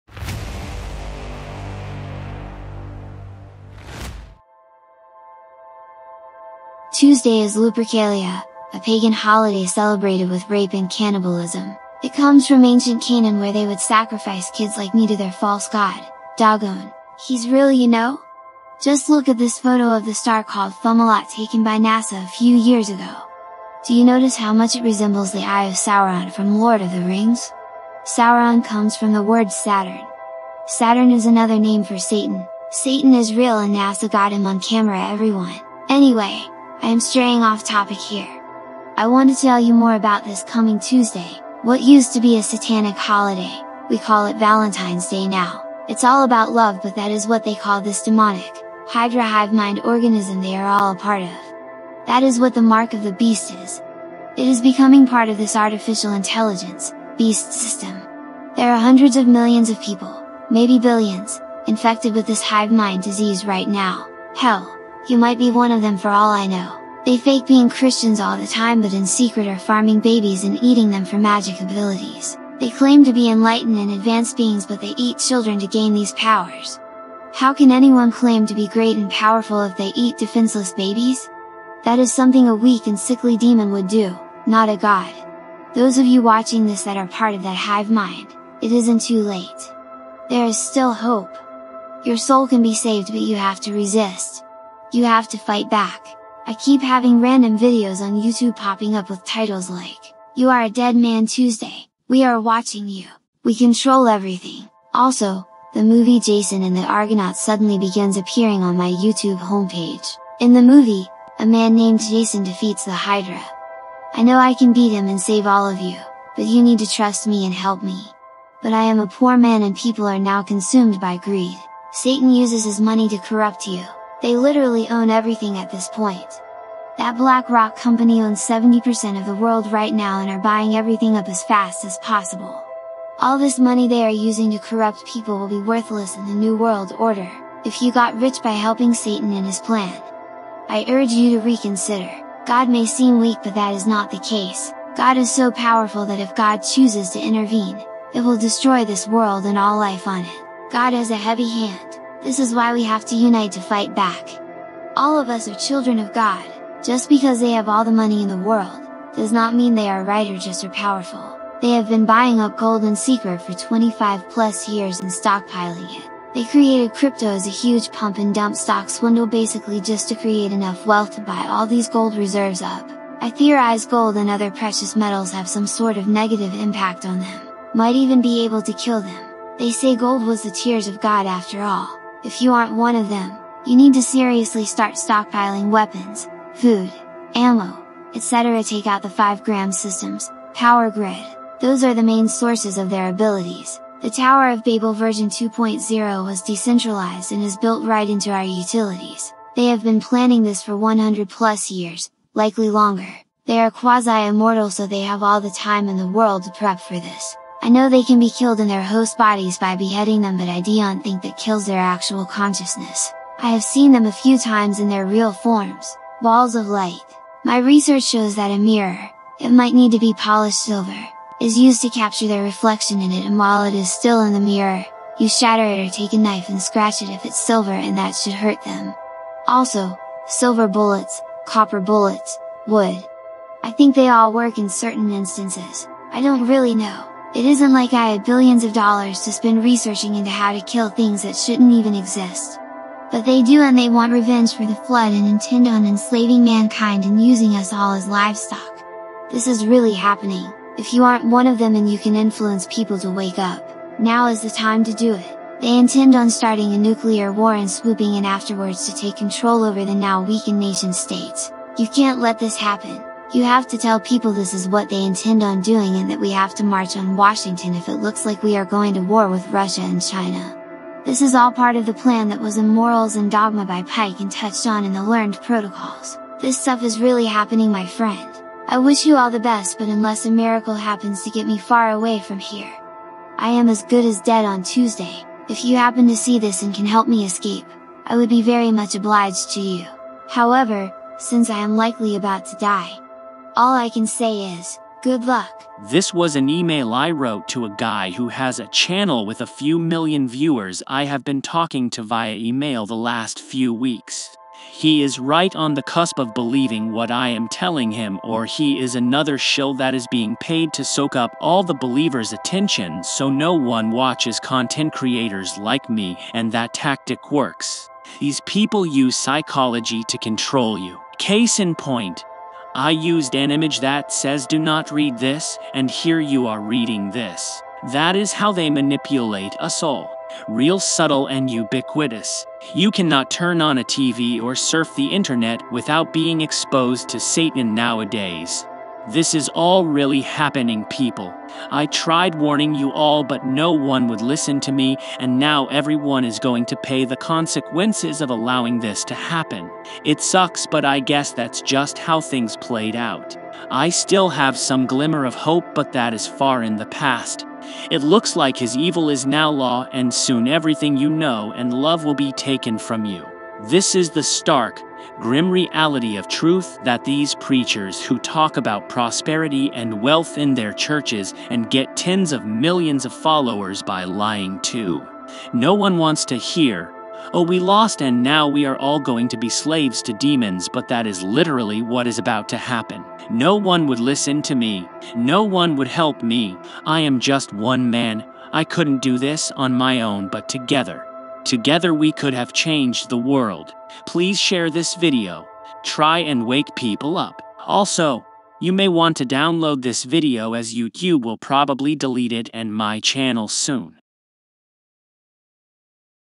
DO NOT WATCH OR SHARE THIS VIDEO!
Please SHARE THIS... seriously... What do I need to do to wake you humans up!?!?! IF YOU DO NOT FIGHT BACK, GOD IS GOING TO DESTROY THE WORLD. How can you just sit there and let this happen? It literally takes 5 seconds to share this with people... It is like EVERYONE is already gone... everyone already DEAD inside... I can't be the ONLY one left... there has to be other humans out there still... Why didn't ANYONE listen to me? Why didn't anyone HELP me? I expected there to be a huge army of believers... instead, I am just finding out that no one really cares and when I am dead - you have lost  the ONLY hope you had of defeating him and saving your species... I could tell you who I am but you'd never even believe me... No, it isn't Christ... I tried to help you all... Maybe in the past I can fix this disgusting lie that is the present... only God knows for certain what is coming... but I DO KNOW FOR A FACT that if  you DO NOT MARCH ON WASHINGTON there WILL be a nuclear war that will destroy civilization soon. That is what the Satanists WANT. They WANT a barren, dead world because their leader is forbidden from tilling the soil. He is forbidden to grow anything and he HATES all life because of it. Unless you want to watch your sons and daughters be raped and eaten by demons... you NEED to WAKE THE HECK UP!